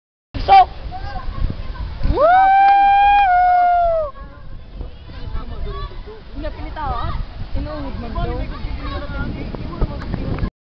A boy Made a Precise Dive at Pinusilan Pool
The young boy at the video is unknown? but he's from Magtaon, Mapanas N. Samar. He dive from a rock which 9 meters estimated  tall from the water. This was taken at Pinusilan Natural pool, during  March 31, 2015.